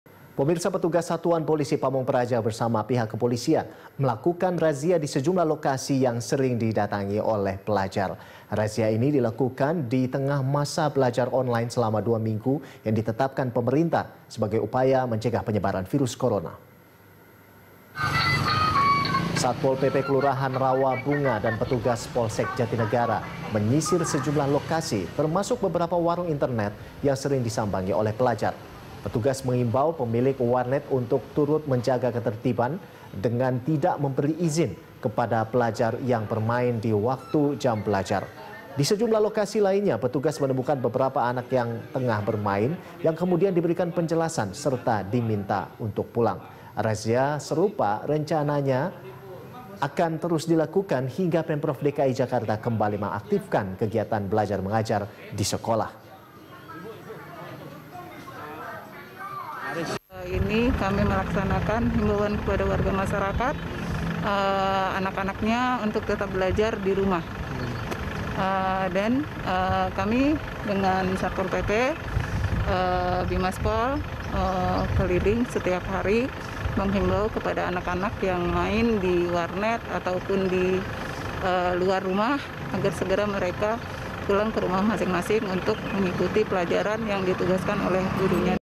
Masa Belajar Online, Satpol PP Razia Pelajar di Luar Rumah
Satpol PP bersama kepolisian melakukan razia di sejumlah lokasi yang sering didatangi pelajar. Razia ini dilakukan di tengah masa belajar online selama 2 minggu yang ditetapkan pemerintah sebagai upaya mencegah penyebaran virus corona.